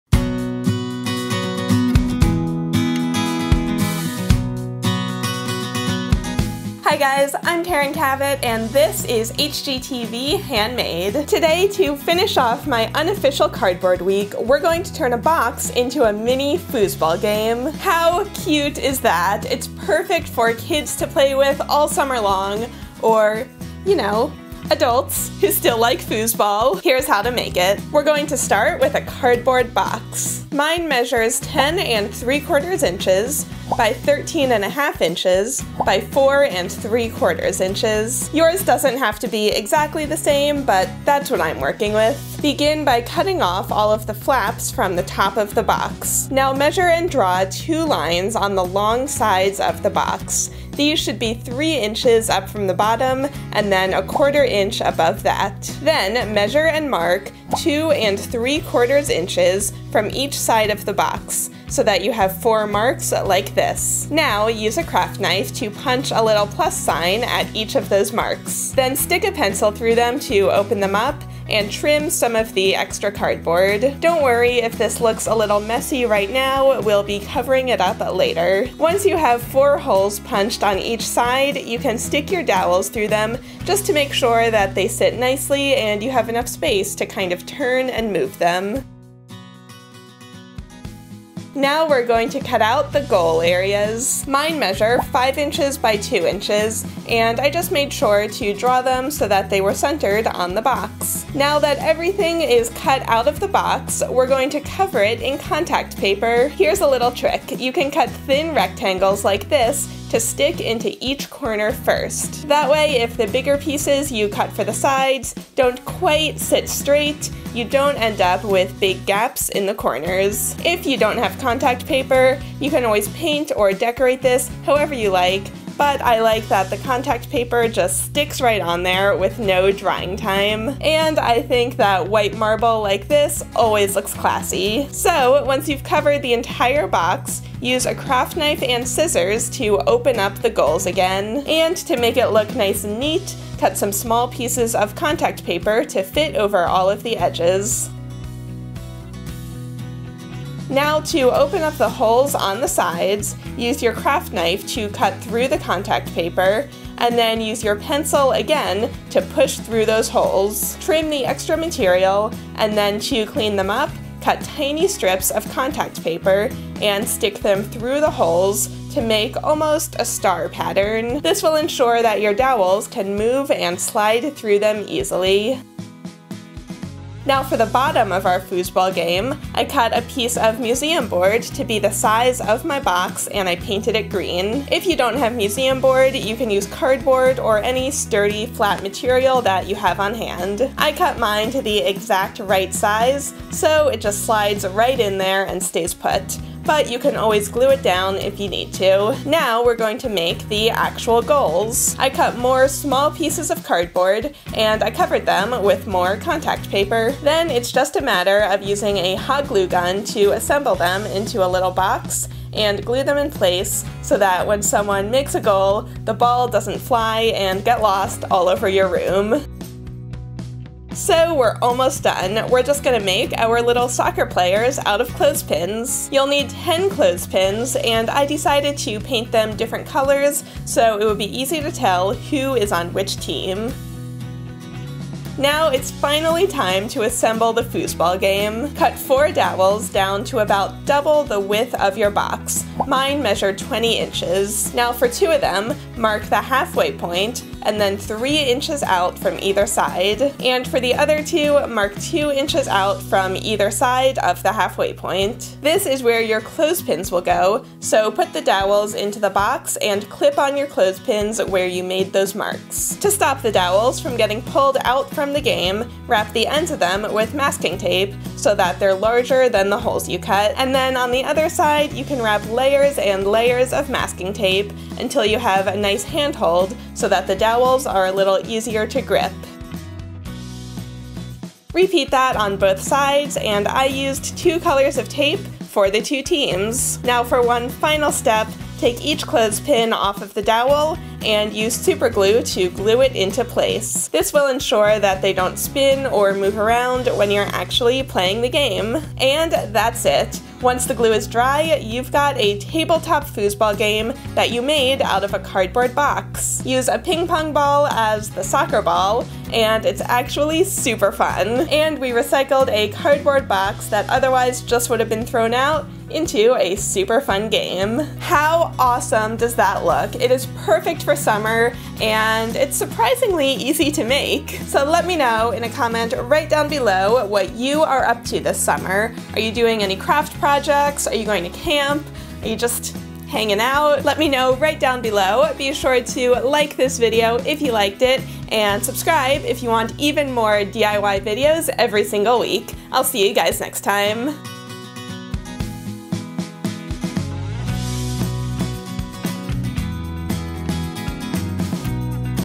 DIY Tabletop Foosball Game! - HGTV Handmade
This tabletop foosball game is really easy to make and perfect for kids and adults to play with all summer!